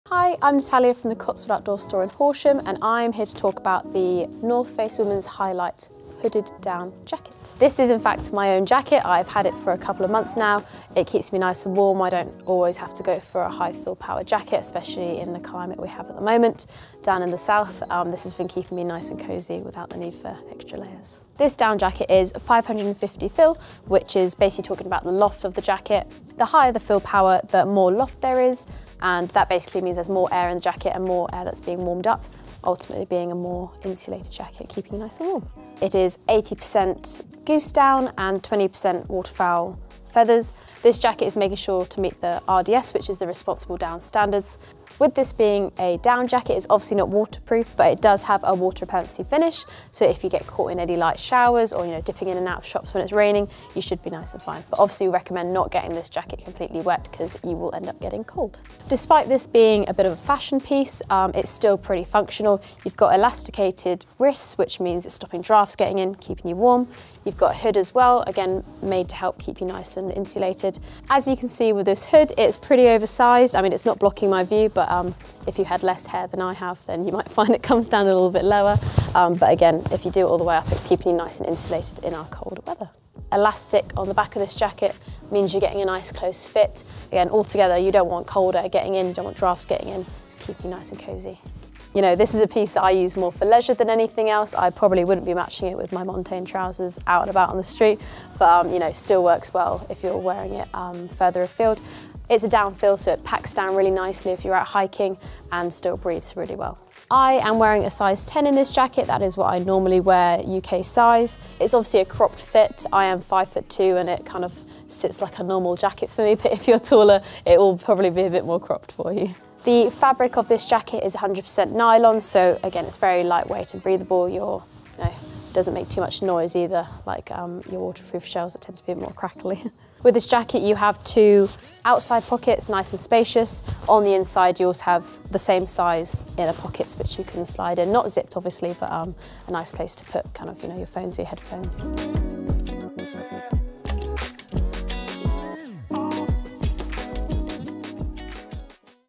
The North Face Hyalite Down Hoodie - Womens Expert Review [2023]
Hear from Natalya, our Horsham store expert reviewing the The North Face Hyalite Down Hoodie.

Product details
Materials:
100% Nylon
Durable Water-Repellent (DWR) Finish
Insulation:
80% Goose Down
20% Waterfowl Feathers
RDS Certified
Clean-finish hem with partial rear elastic binding
Two Vislon zippered hand pockets
Exposed Vislon centre front zip
Internal elastic cuffs
Quilted Stitching
Water-repellent
Attached hood
Lightweight
Brand logo
Breathable
Packable
Fit: Cropped
Product information
Don't be fooled by its street-inspired design or cool cropped fit. The Hyalite Down Hooded Jacket from The North Face is built to perform on the trail. It's loaded with 550FP of responsibly-sourced down, which keeps warm through the cold without bogging you down with excess weight. It's also got plenty of the hiking features you'd expect too: an attached hood to block out chills, handwarmer pockets and elastic cuffs to lock in warmth.